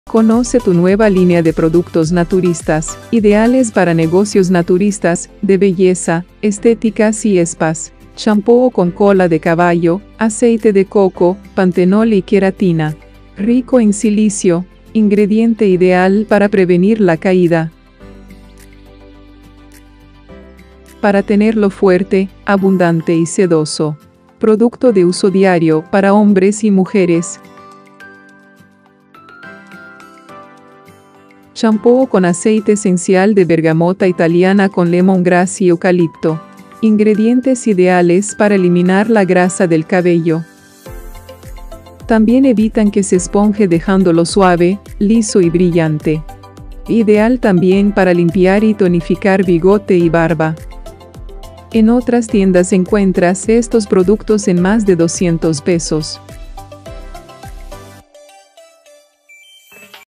2 champús naturistas ¡Conoce nuestra nueva línea de productos de belleza! 🤩✨
Nos han llegado nuevos productos naturistas; champús, cremas, geles y pomadas. Toda una línea de productos con variedad de propiedades útiles para tu cuerpo, piel y cabello. 2 champús naturistas; Cola de Caballo y Bergamota Italiana ¡Conoce nuestra nueva línea de productos de belleza!  Solo en Tiara by Grupo Julio's 🤩✨

¡COMPRA TUS PRODUCTOS AQUÍ! ⤵️⤵️⤵️